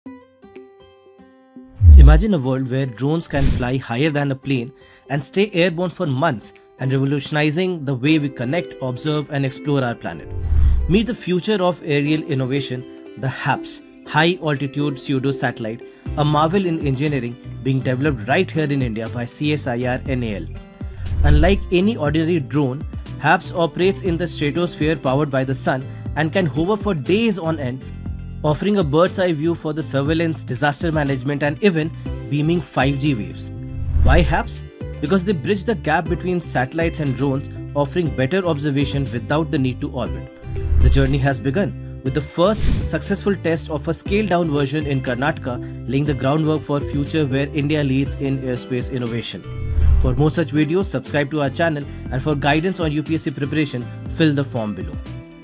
Unlocking the Potential of HAPS: A Game-Changing Breakthrough
CSIR-National Aerospace Laboratories (NAL) scientists have successfully tested an unmanned aerial vehicle, High Altitude Pseudo Satellite (HAPS). Learn about the High Altitude Pseudo Satellite (HAPS), which is seen as a huge technological breakthrough and finds application in various fields, from surveillance to beaming 5G waves. HAPS also have the potential to overcome the shortcomings of satellites.

1. What is HAPS?
2. Why HAPS is considered a huge technology breakthrough
3. How does a HAPS function?
4. What are the applications of HAPS?
5. How India is leading in this technology?